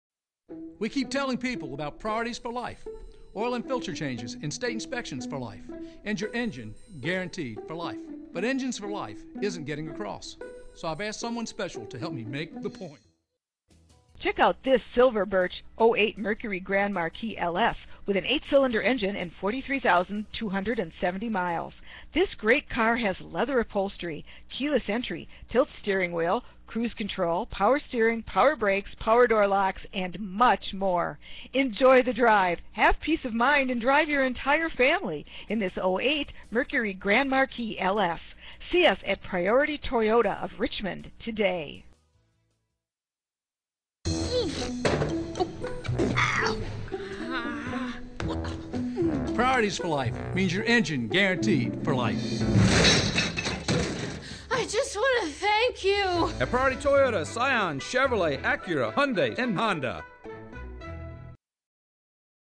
2008 Mercury Grand Marquis LS Priority Toyota of Richmond
2008 Mercury Grand Marquis LS- You will love this Silver Birch Clearc 2008 Mercury Grand Marquis LS, equipped with an 8 Cyl. engine with 43,270 miles. Enjoy this great car with features like Leather Upholstery, Keyless Entry, Tilt Steering Wheel, Cruise Control, Power Steering, Power Brakes, Power Door Locks, and much more. Enjoy the drive, have peace of mind, and drive your entire family in this 2008 Mercury Grand Marquis LS. See us at Priority Toyota of Richmond today!
Miles-  43,270
VIN- 2MEFM75VX8X623956

Priority Toyota of Richmond
2000 Walthall Center Dr.